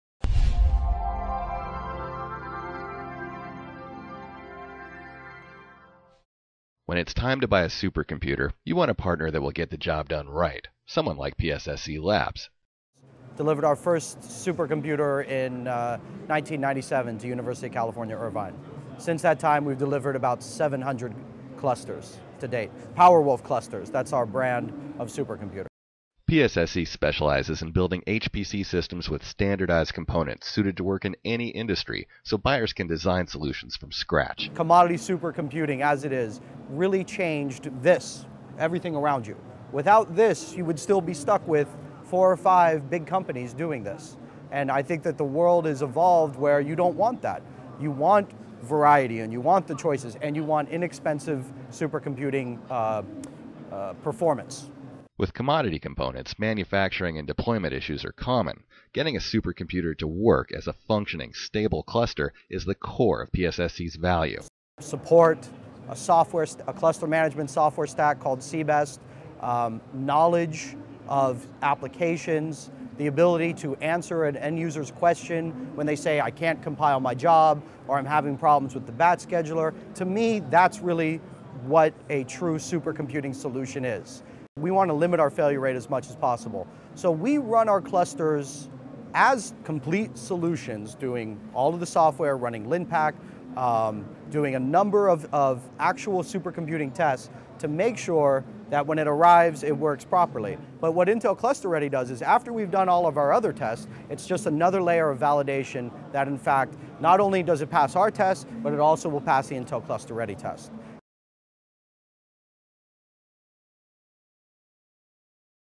PSSC Labs & Intel® Cluster Ready
Alex Lesser from PSSC Labs talks about supercomputing, PowerWulf Clusters, and the choice of lower cost HPC clusters with Intel Cluster Ready.